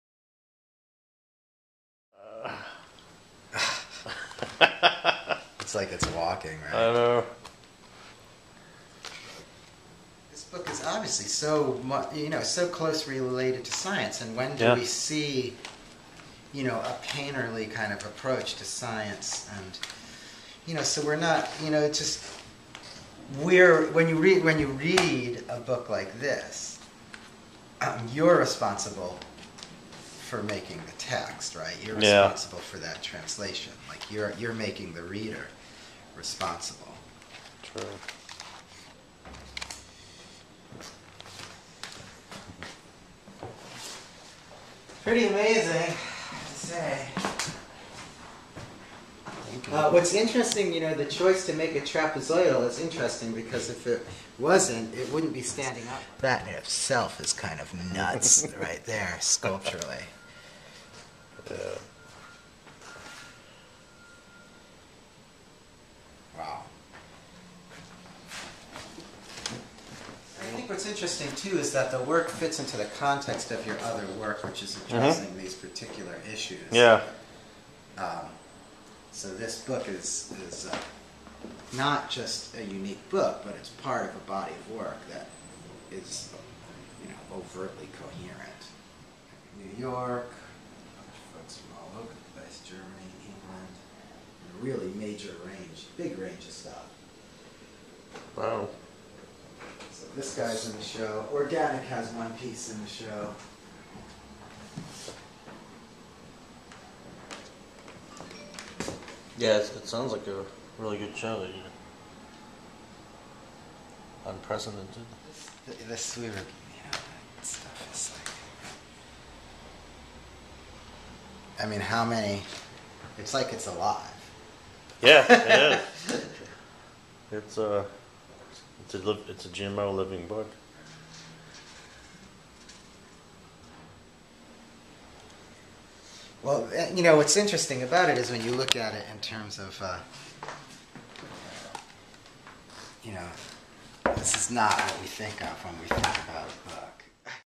LEYBA's 12th hand made book "TRAPezoid godSANTO CODEX"
Marshall Weber Artist, Directing Curator Booklyn Artists Alliance looks at Steven Johnson Leyba's 12th hand made book "TRAPezoid godSANTO CODEX ailmentarius"